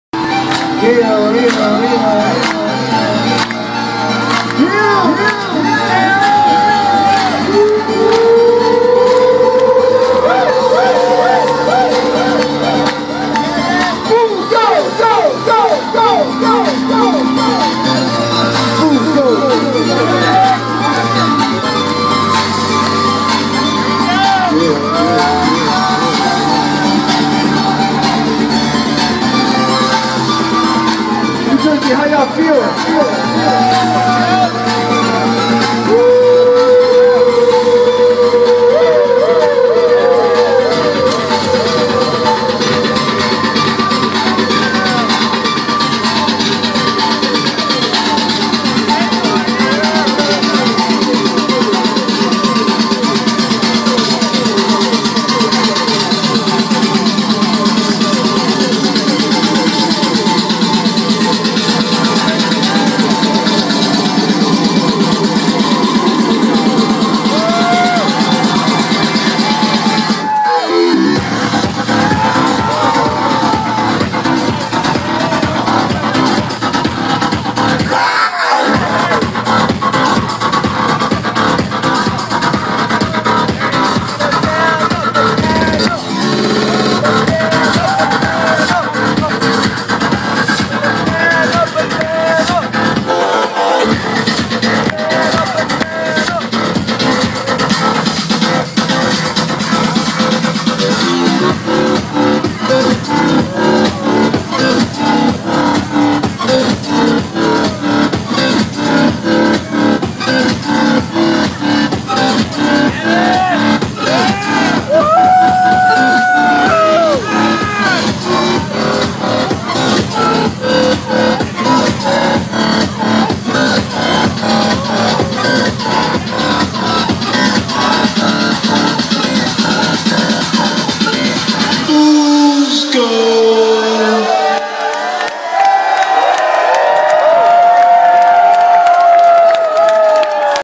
A-Trak - ID? (House Party tour, Asbury Lanes)
WATCH IN HD! A-Trak and Flosstradamus threw down killer sets in... a bowling alley?  Yep.  Before the show started for about an hour and half they walked around talking and taking pictures with everyone - really down to earth guys